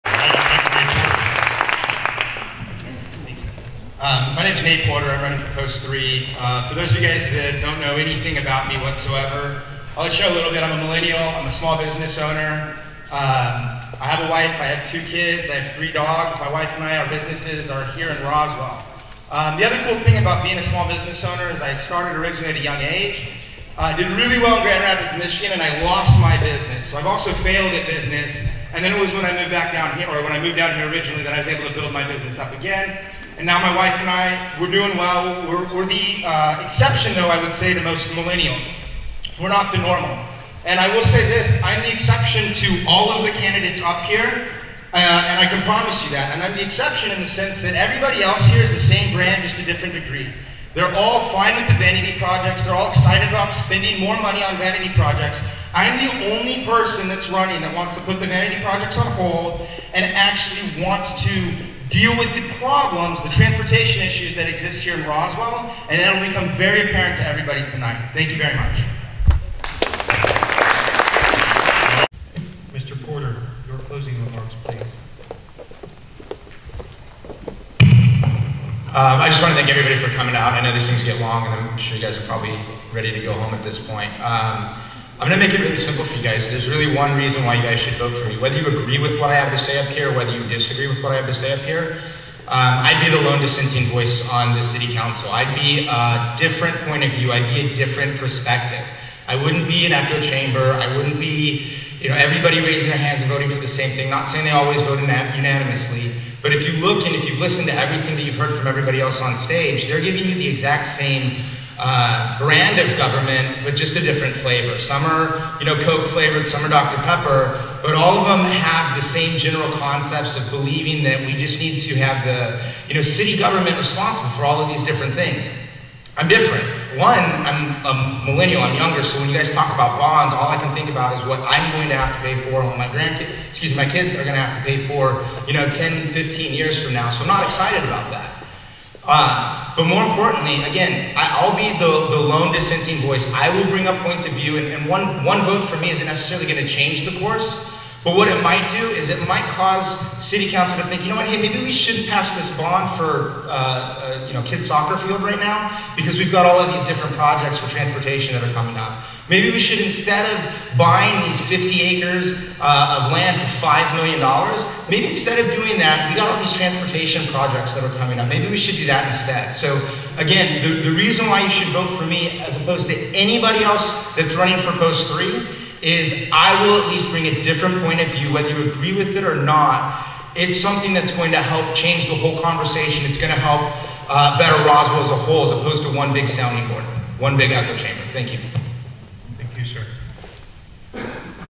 Nate Porter – Roswell, GA City Council Candidate, Post 3 10/29/15
Roswell City Council Post 3 candidate Nate Porter addresses voters at the RoswellNEXT 2015 Candidate Forum at High Meadows School on Thursday, October 29, 2015.